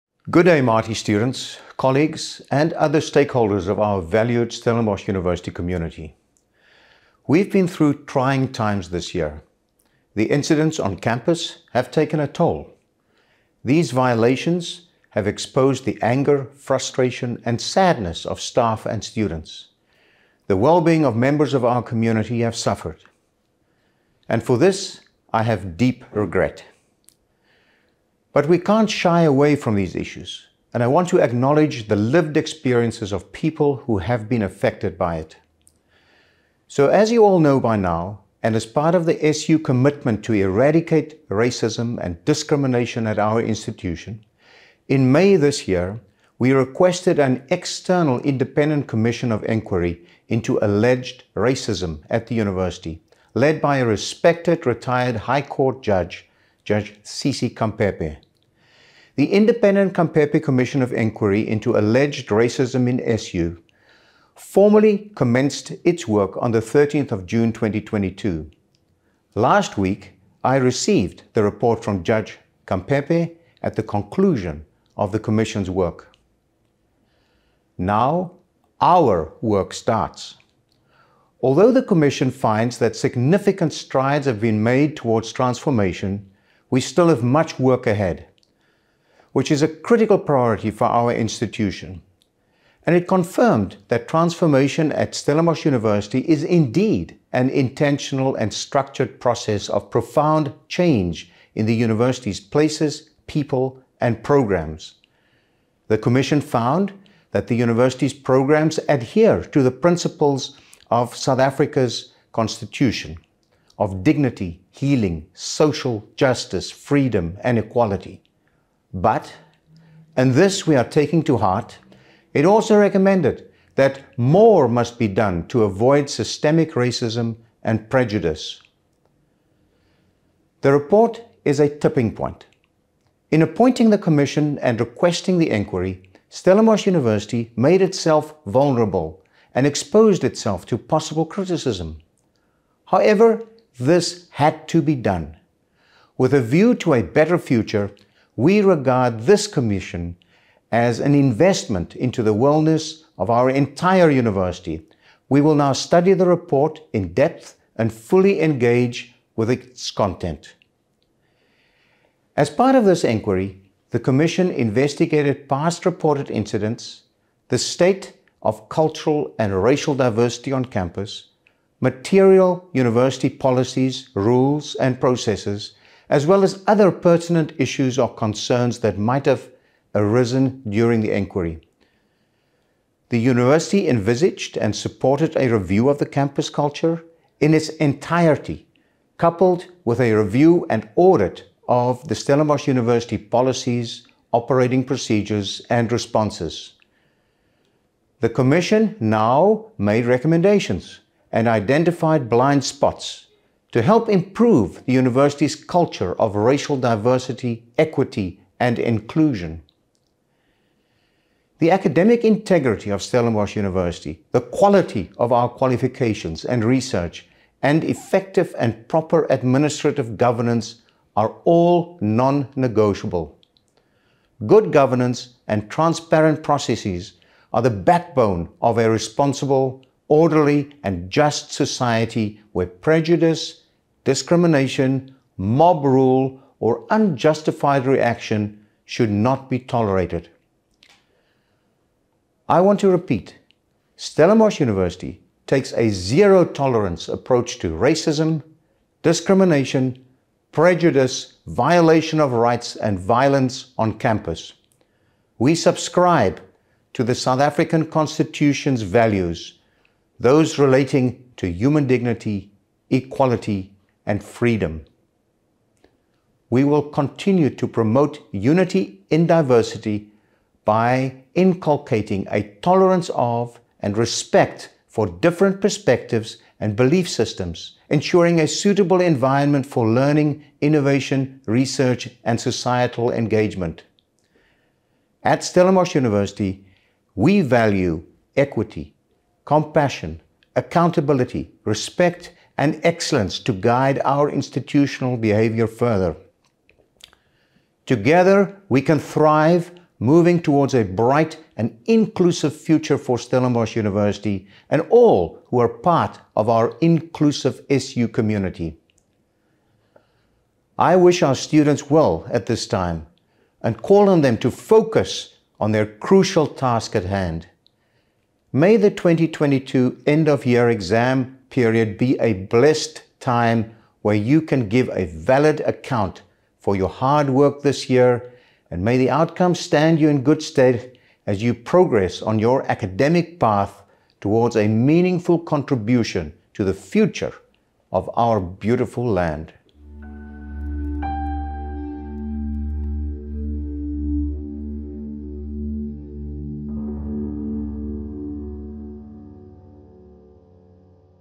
Khampepe independent commission report
The Rector and Vice-Chancellor of Stellenbosch University, Prof Wim de Villiers, has received the report of the Khampepe independent commission of enquiry into alleged racism at Stellenbosch University (SU) from Judge Sisi Khampepe. The independent Khampepe commission of enquiry into allegations of racism at SU, led by respected retired Judge Sisi Khampepe, formally commenced its work on 13 June 2022. The University leadership requested this external independent enquiry into alleged racism at the University in May this year as part of SU’s commitment to root out racism and discrimination.